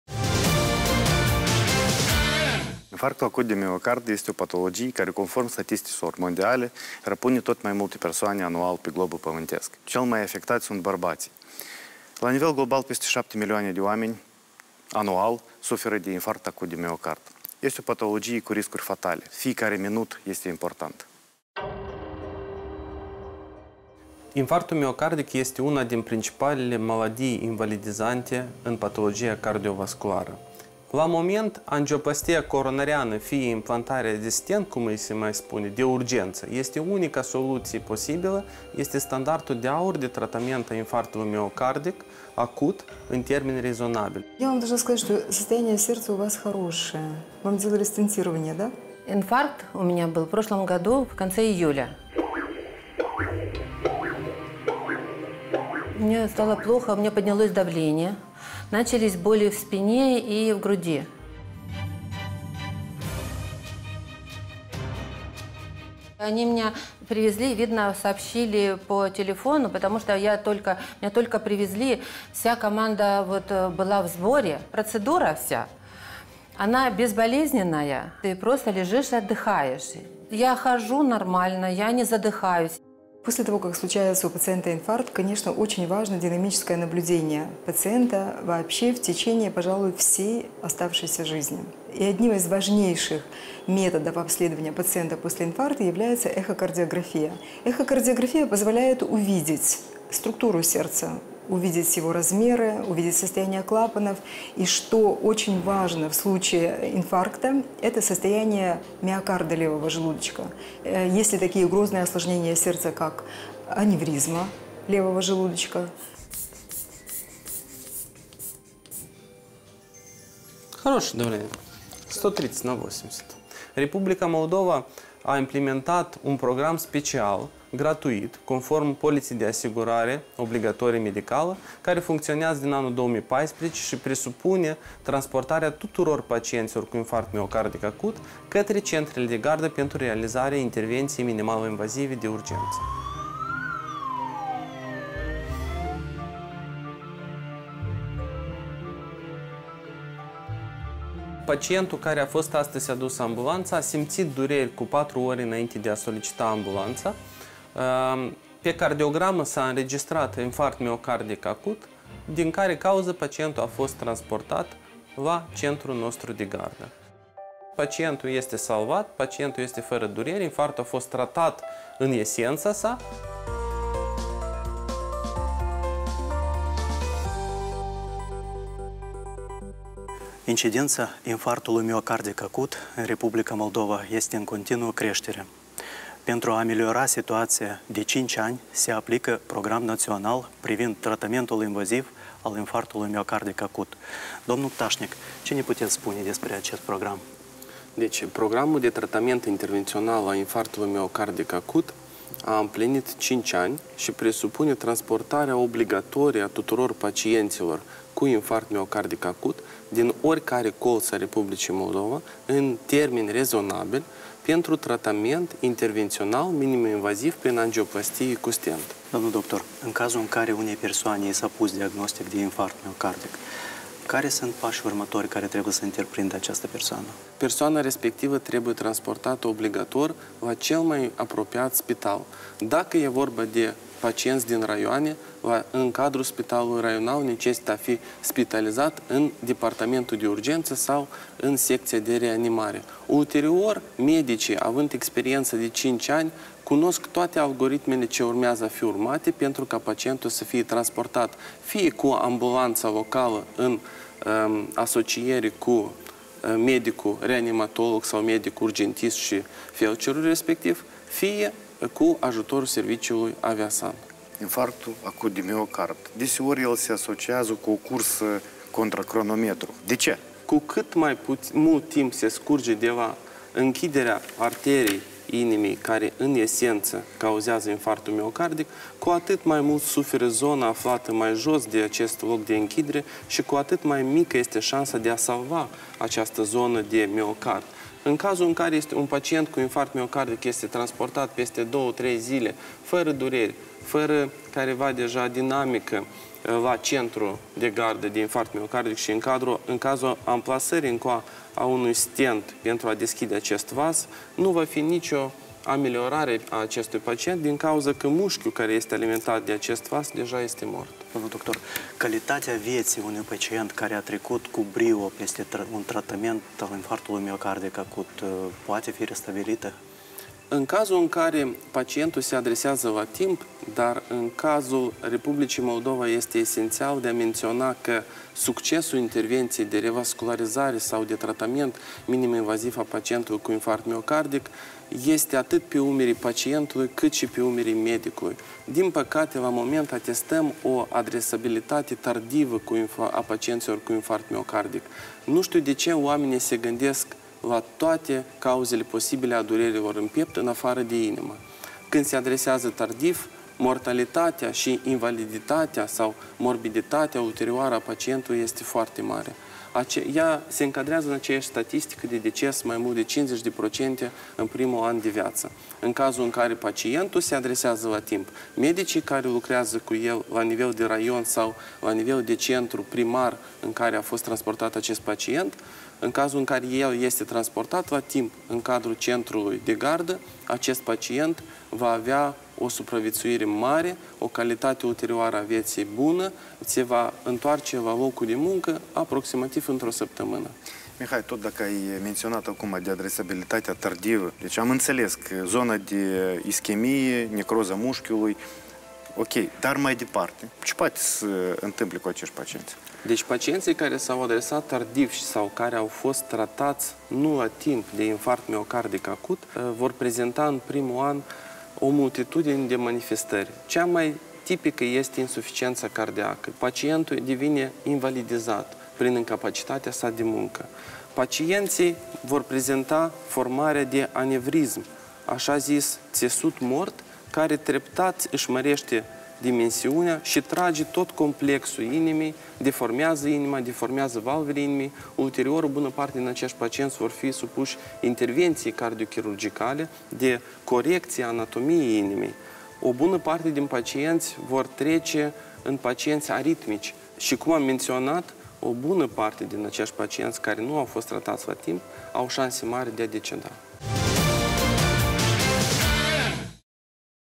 Infarctul miocardic acut: diagnostic și tratament
Anual circa 7 milioane de oameni sunt răpuși de infarctul miocardic. Aflați despre standardul de aur în tratarea acestei maladii de la medicul cardiolog-intervenționist Medpark,  Mihai Tașnic.

Programări la ☎️ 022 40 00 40 (disponibil 24/24).